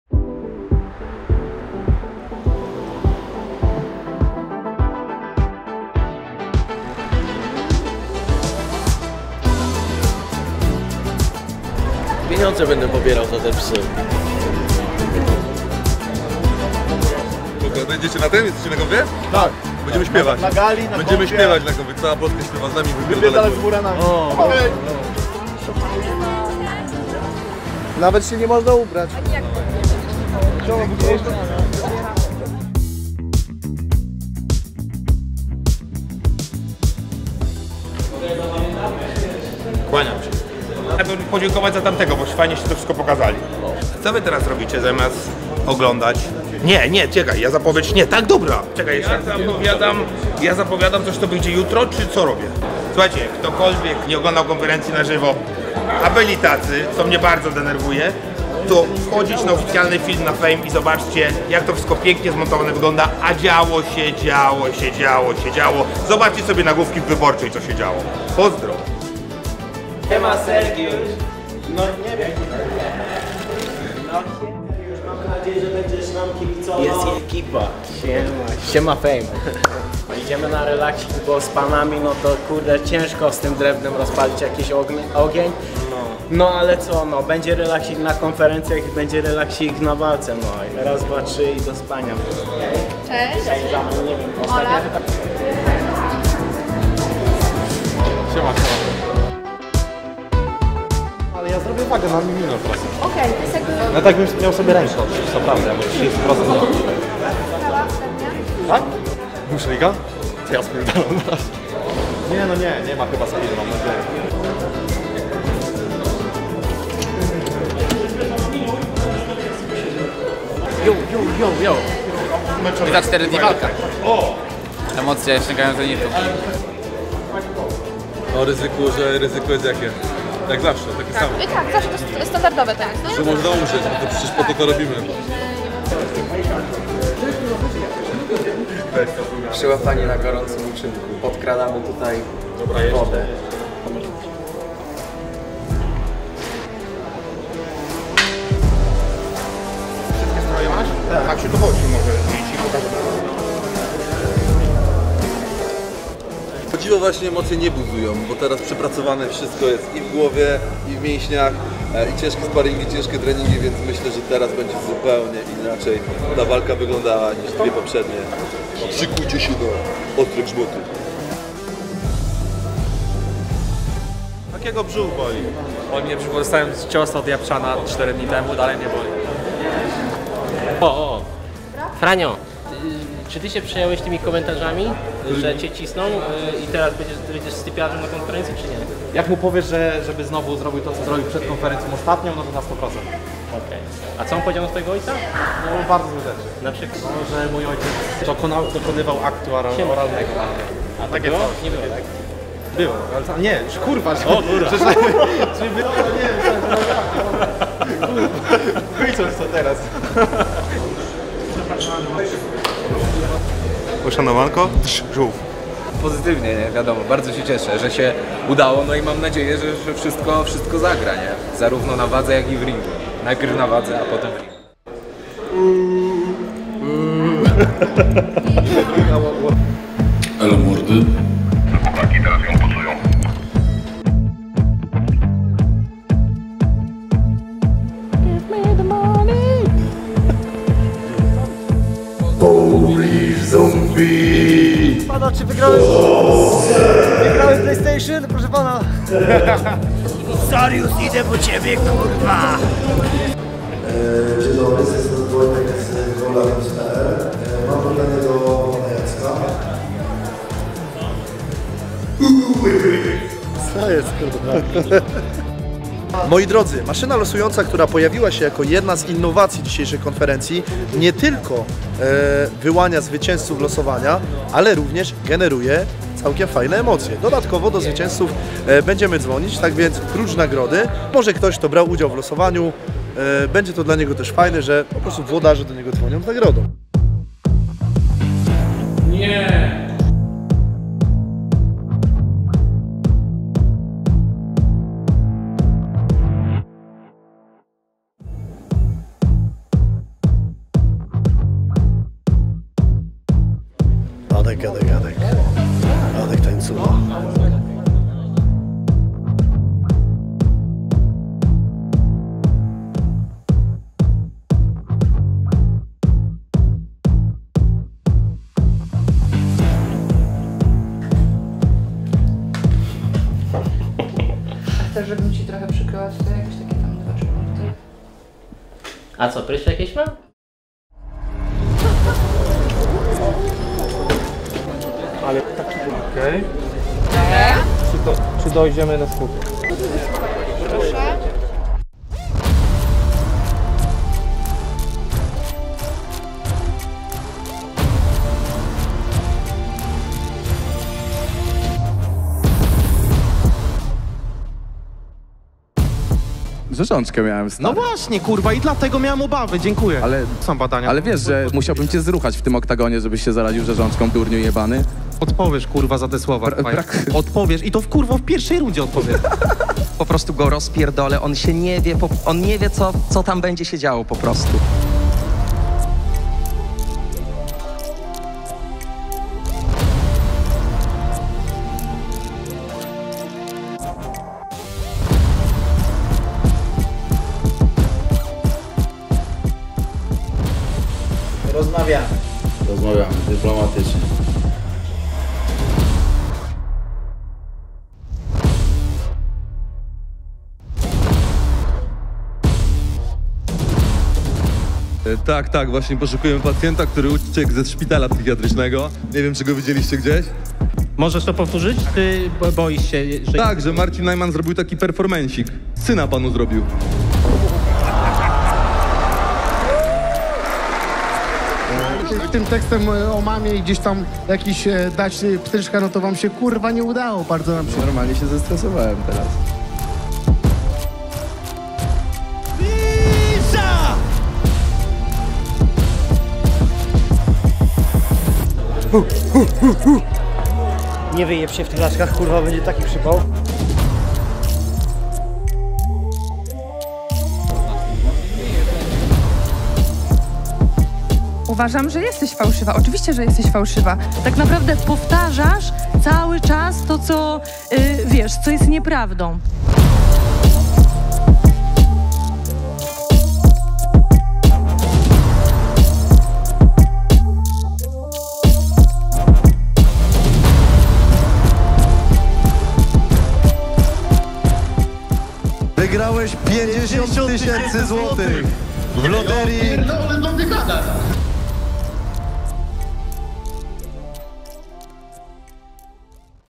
FAME 15: Relacja z II Konferencji
GALA FAME 15 JUŻ W PIĄTEK! 🔥 KUP PPV / BILET

Piękna to była Konferencja - zostanie w naszych serduszkach na zawsze ♥️
Piotrek Szeliga miał amerykański luz, dlatego Franio mógł ze spokojem przemieszczać się na terenie DoubleTree by Hilton 😅😅😅

Sprawdźcie, co jeszcze zobaczyła nasza kamera! 🔥

FAME 15
26 sierpnia 2022, 20:00 / Atlas Arena Łódź